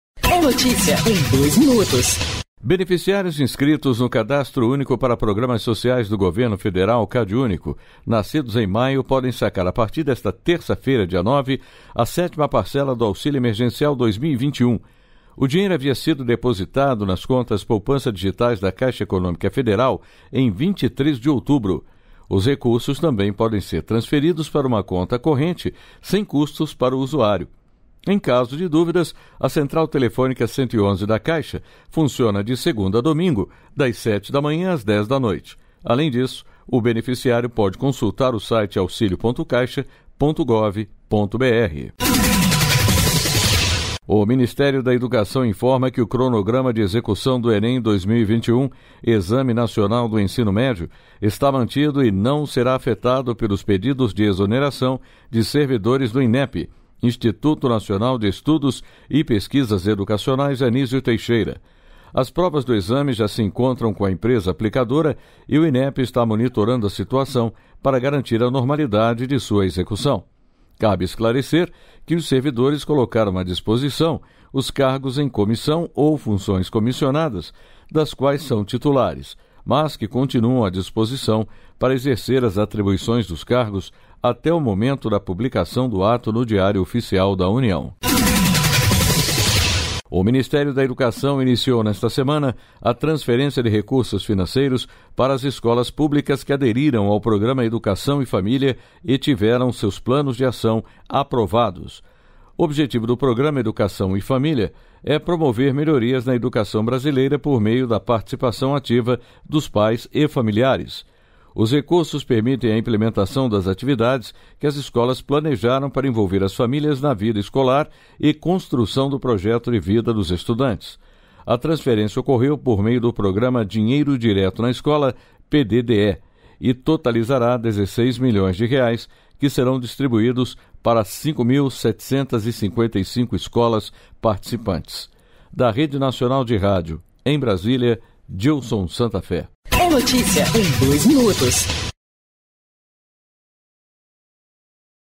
09/11/2021 - BENEFICIÁRIOS NASCIDOS EM MAIO PODEM SACAR AUXÍLIO EMERGENCIAL | É Notícia em 2 Minutos
E mais: MEC informa que o cronograma de execução do Enem 2021 está mantido;

Ministério da Educação inicia transferência de recursos financeiros para escolas públicas aprovadas no Programa Educação e Família.

Beneficiários inscritos no Cadastro Único para Programas Sociais do Governo Federal (CadÚnico) nascidos em maio podem sacar, a partir desta terça-feira (9), a sétima parcela do auxílio emergencial 2021. O dinheiro havia sido depositado nas contas poupança digitais da Caixa Econômica Federal em 23 de outubro.

Os recursos também podem ser transferidos para uma conta corrente, sem custos para o usuário.

Em caso de dúvidas, a central telefônica 111 da Caixa funciona de segunda a domingo, das 7 horas da manhã às 10 horas da noite. Além disso, o beneficiário pode consultar o

Xxxxxxxxxxxxxxxxxxxxxxx

O Ministério da Educação informa que o cronograma de execução do Enem 2021, Exame Nacional do Ensino Médio, está mantido e não será afetado pelos pedidos de exoneração de servidores do Inep, Instituto Nacional de Estudos e Pesquisas Educacionais Anísio Teixeira.

As provas do exame já se encontram com a empresa aplicadora e o Inep está monitorando a situação para garantir a normalidade de sua execução.

Cabe esclarecer que os servidores colocaram à disposição os cargos em comissão ou funções comissionadas das quais são titulares, mas que continuam à disposição para exercer as atribuições dos cargos até o momento da publicação do ato no Diário Oficial da União.

Xxxxxxxxxxxxxxxxxxx

O Ministério da Educação jniciou nesta semana, a transferência de recursos financeiros para as escolas públicas que aderiram ao Programa Educação e Família e tiveram seus planos de ação aprovados .

O objetivo do Programa Educação e Família é promover melhorias na educação brasileira por meio da participação ativa dos pais e familiares.
Os recursos permitem a implementação das atividades que as escolas planejaram para envolver as famílias na vida escolar e construção do projeto de vida dos estudantes. A transferência ocorreu por meio do Programa Dinheiro Direto na Escola (PDDE) e totalizará 16 milhões de reais, que serão distribuídos para 5.755 escolas participantes.

Da Rede Nacional de Rádio, em Brasília, Dilson Santa Fé

registrado em